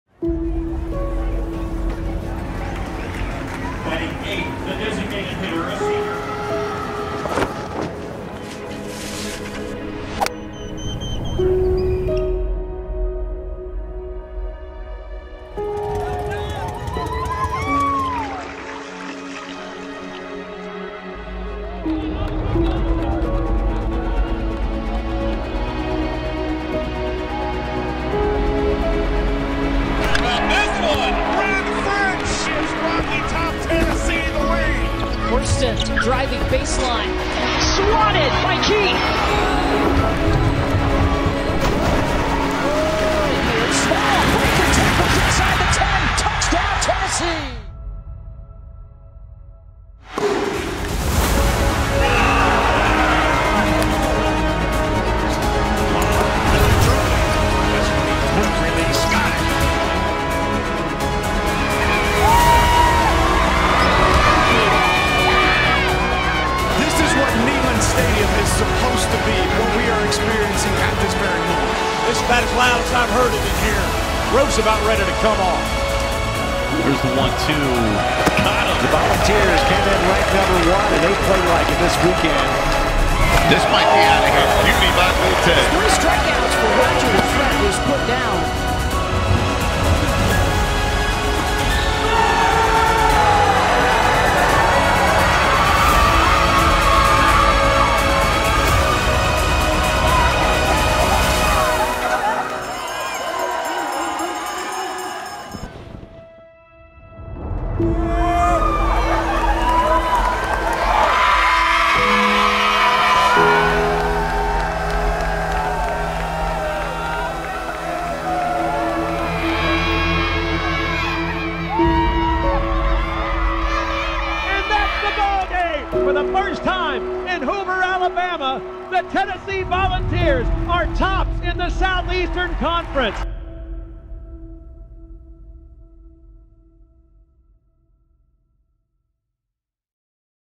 2021-22: A Year to Remember for Tennessee Athletics
Tennessee athletics hoisted five SEC team championship trophies in 2021-22. The Lady Vols soccer team made an impressive run in the SEC tournament where they avenged their loss to Arkansas in the SEC tournament championship to capture the title. The Lady Vols swimming and diving team scored 1,313.5 points enroute to their second SEC title in three years. The Tennessee men's basketball team defeated Texas A&M in the SEC tournament championship to capture their first title since 1979. The Tennessee baseball team displayed pure dominance throughout SEC play, winning the regular season title and the SEC tournament championship.